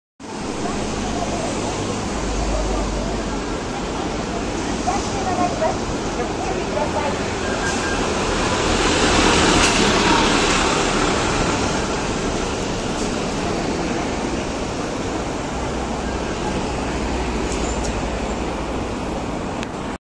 阪神バス 三菱ふそうエアロエース 582 阪神三宮駅付近にて
主に三宮線で運用されます